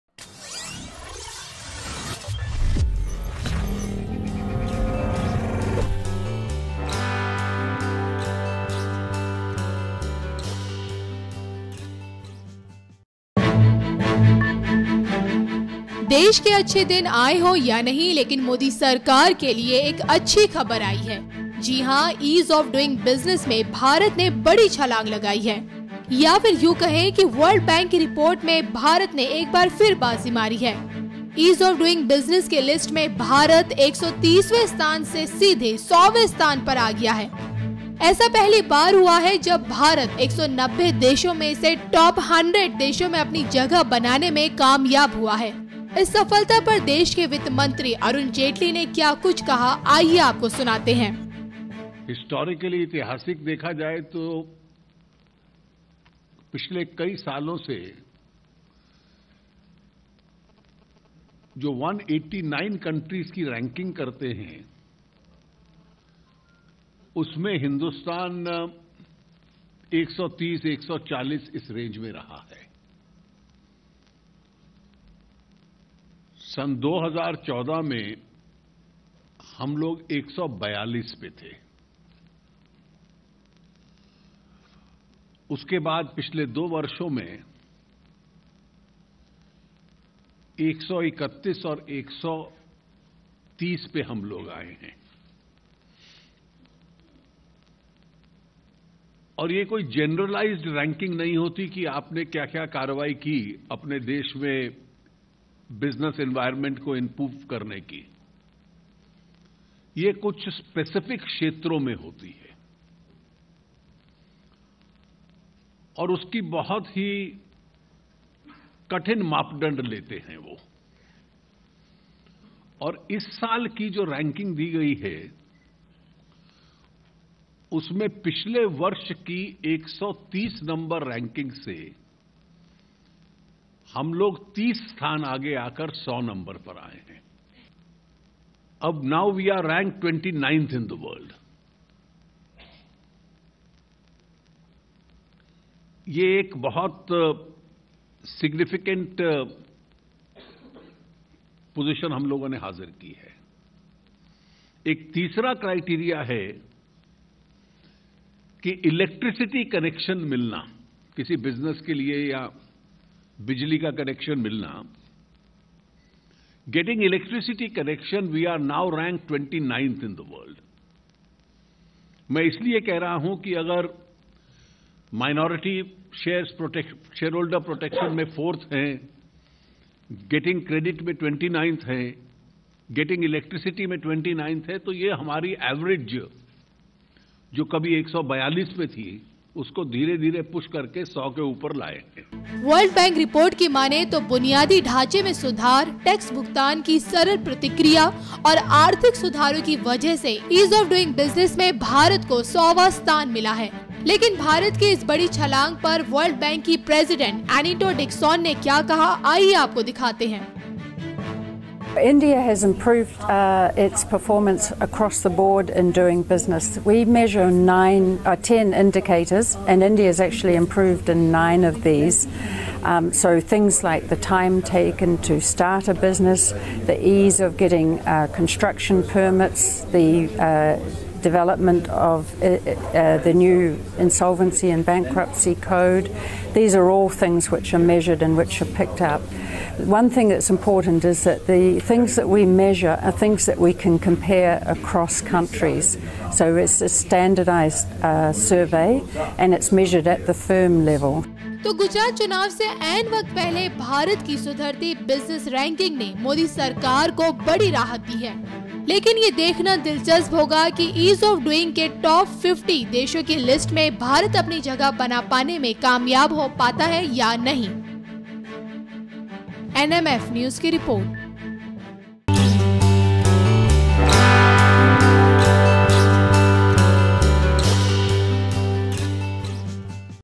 नज़र आने लगा है Modi Effect , Ease Of Doing Business में India ने मारी लंबी छलांग
According to World Bank report, India jumps 30 places in ease of business ranking. Last year, India had risen one place to 130 among the 190 economies assessed.